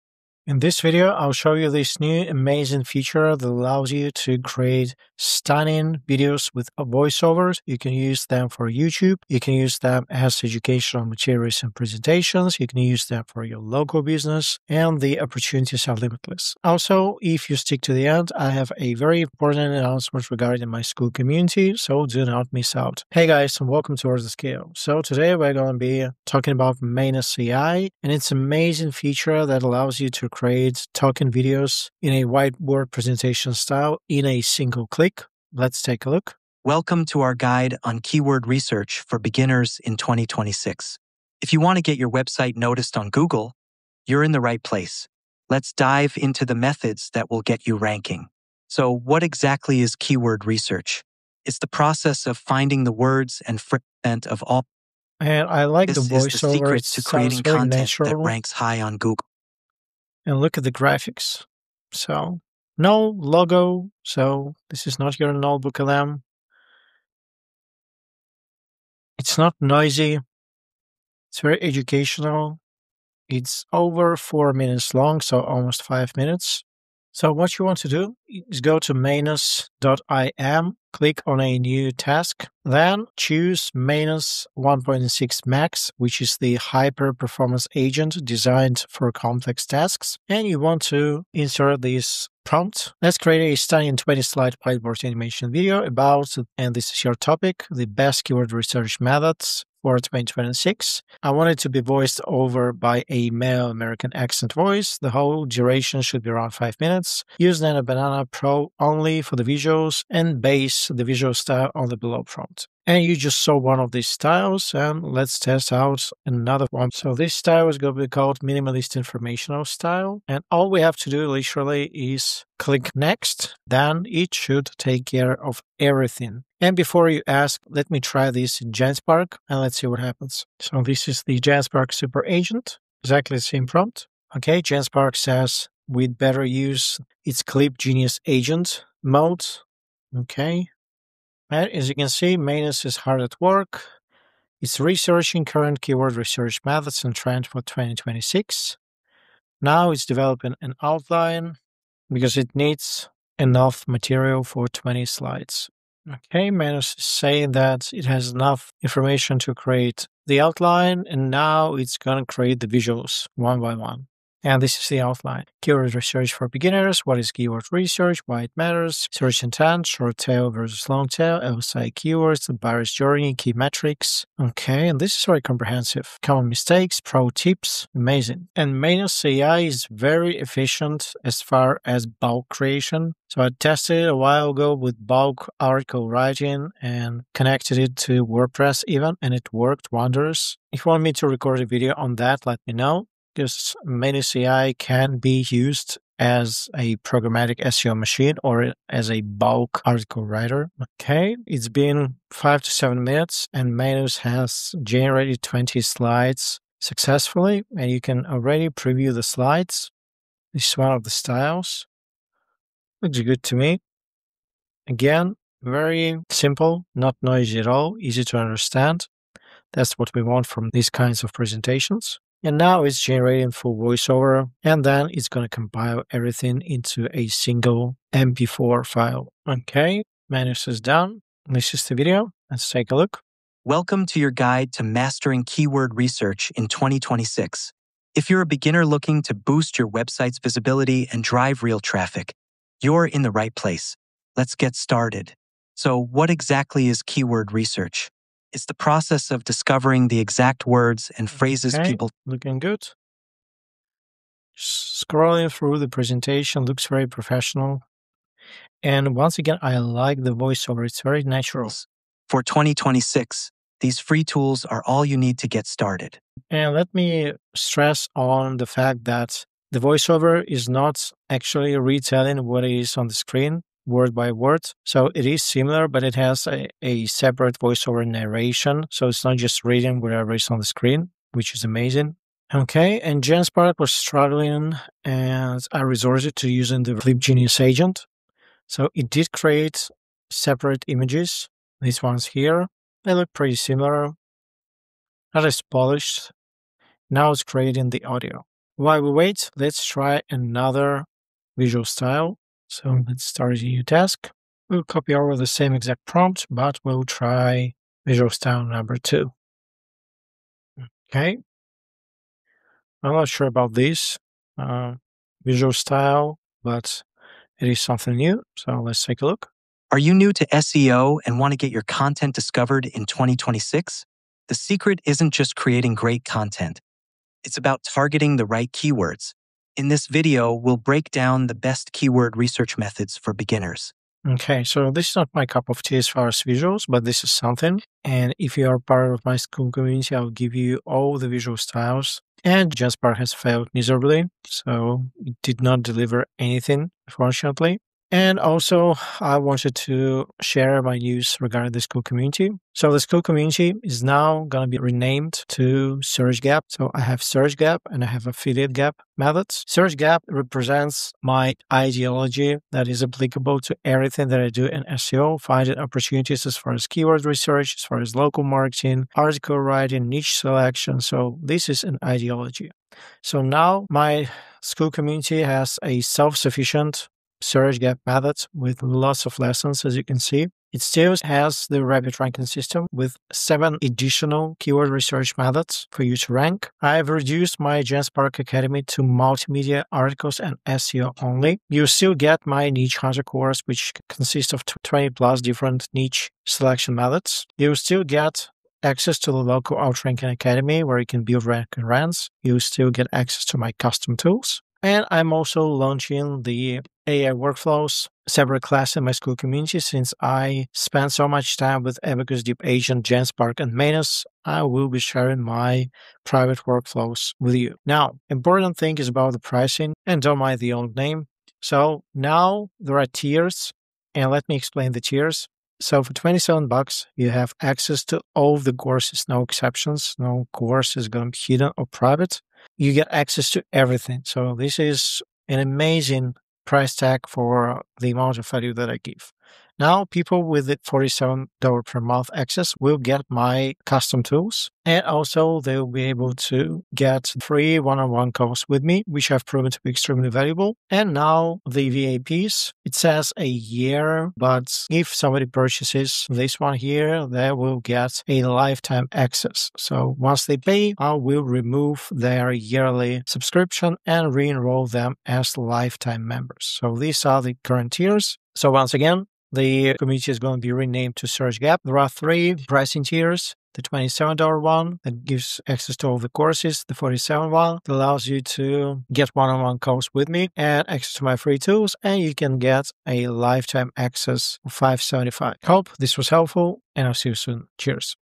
This NEW AI Tool Creates AMAZING Talking Whiteboard Animation Videos (20 slides)
Talking whiteboard animation videos are now easier than ever with the new Manus AI agent. In this video, I show you how to generate 20-slide educational videos with professional voiceovers in just one click.

📚 TIMESTAMP CHAPTERS:
0:00 Introduction to Whiteboard AI
1:20 Setting up the Manus AI Prompt
2:10 Manus AI vs JenSpark Comparison
2:45 Reviewing the AI Generated Outline
3:52 The Final Video Result & Voiceover
5:52 Big Announcement: Search Gap Community
7:00 New Pricing Tiers & Lifetime Access

❓ FAQ:
Q: What is the best AI for whiteboard animation?
A: Manus AI (specifically the 1.6 Max agent) is currently one of the best for creating multi-slide whiteboard videos with integrated voiceovers.

Q: Can I make whiteboard videos for free?
A: While Manus AI has paid tiers, it offers high-performance agents like the 1.6 Max that can generate full 5-minute videos from a single prompt.

Q: Does the AI voiceover read exactly what is on the screen?
A: No, the voiceover provides a separate narration that complements the visuals, making it feel more professional and less repetitive.